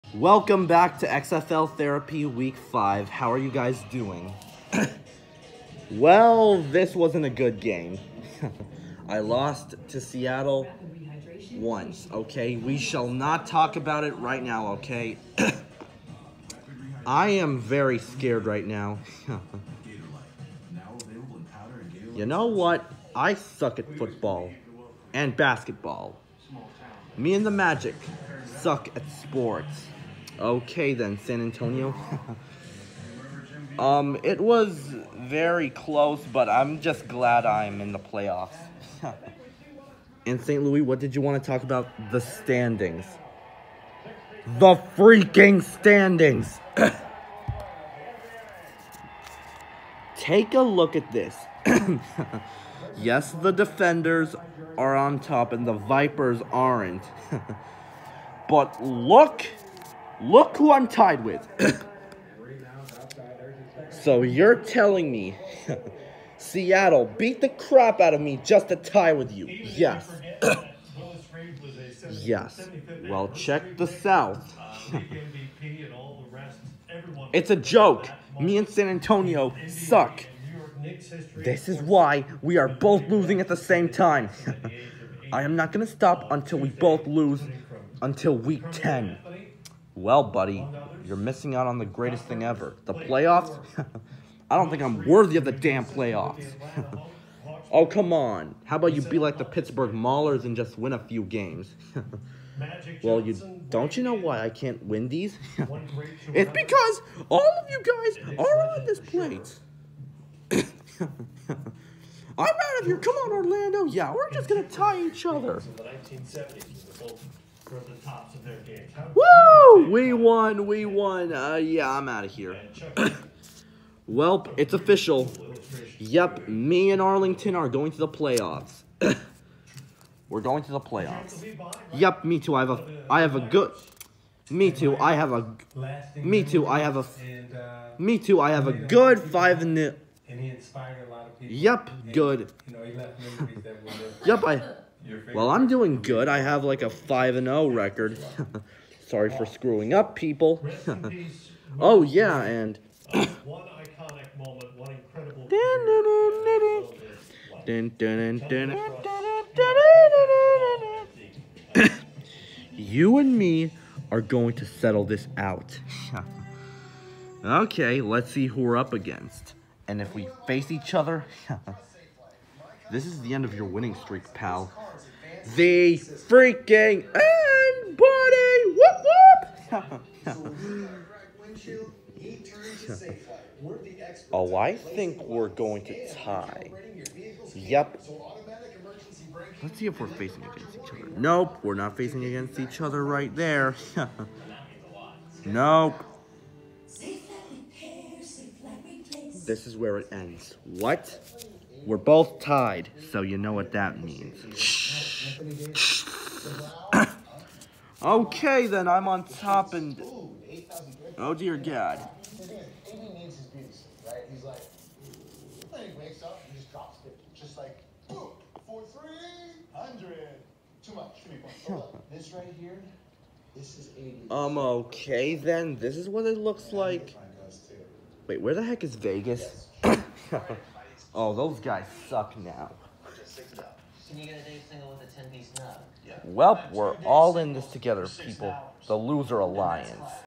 XFL Therapy week 5 + Playoff Announcement
Roughnecks: You lost but at least your not the Texans Buddy.

Battlehawks: I believe in you. Win more games your up against Vegas so it can’t go wrong.

Guardians: Why do I keep talking about you Orlando Huh? You can’t even put it together.

Brahmas: Okay San Antonio only 5 more weeks and you can move up a bit to the playoff brackets.

Playoff Contentions so far:

XFL North:
DC Defenders 5-0
St Louis Battlehawks and Seattle Sea Dragons 3-2
Vegas Vipers: Eliminated

XFL South:
Houston Roughnecks 4-1
Arlington Renegades 3-2
San Antonio Brahmas: 34% Chance of making the playoffs.
Orlando Guardians: Eliminated